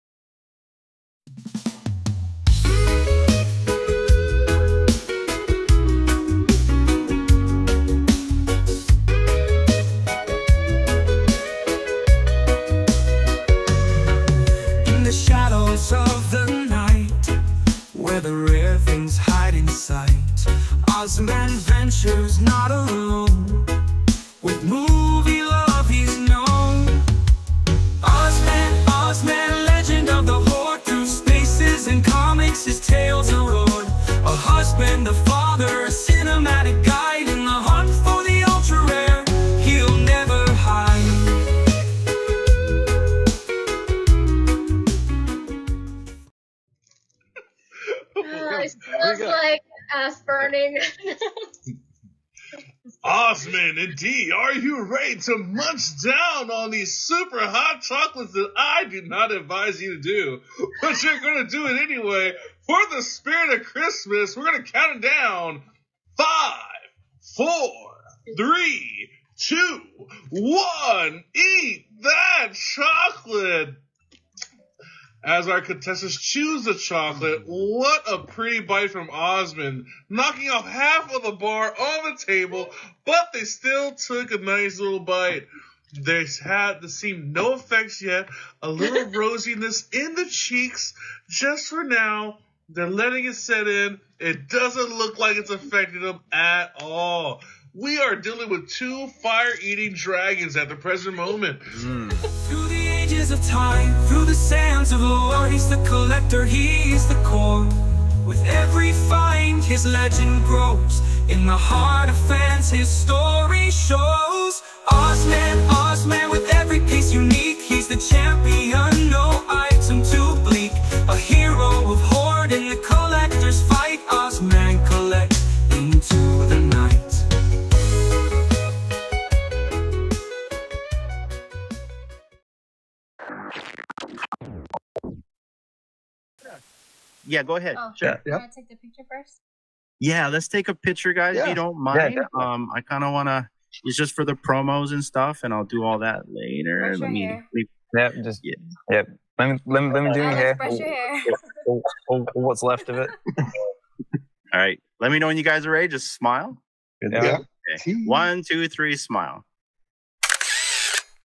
Community Center Stage. OzmanKollects63.
Today's Community Center Stage is focused on OzmanKollects63

Husband, father, president of the Web V Collective and host of "Get To Da Comics" & "X Spaces."  Ultra Rare collector of @veve_official comics.

Check out his YouTube channel & X socials for more coverage on fandom and digital collecting.